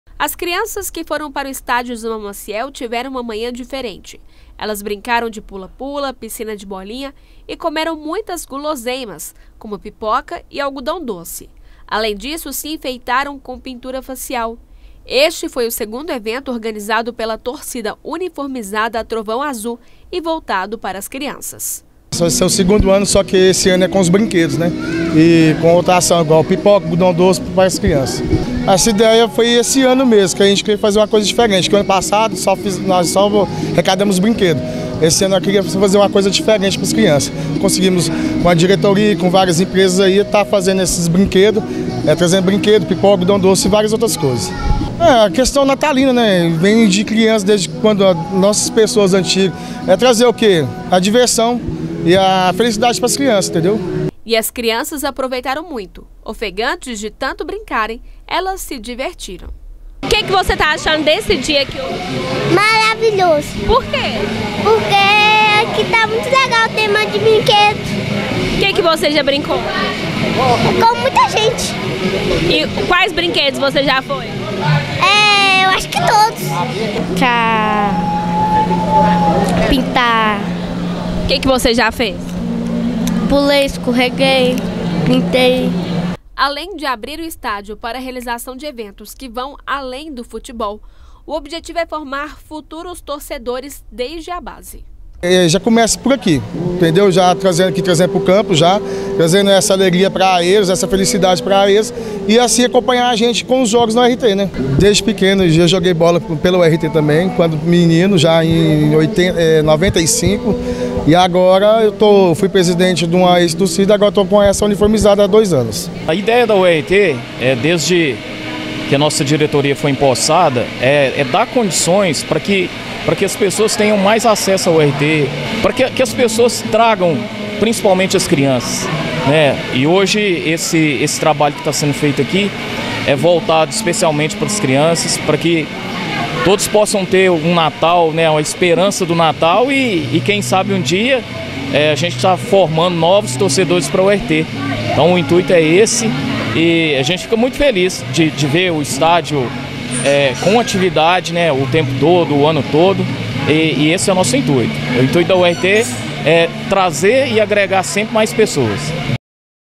NATAL TORCIDA UNIFORMZADA URT 1
Torcida Uniformizada Trovão Azul usa estádio da URT para a diversão de crianças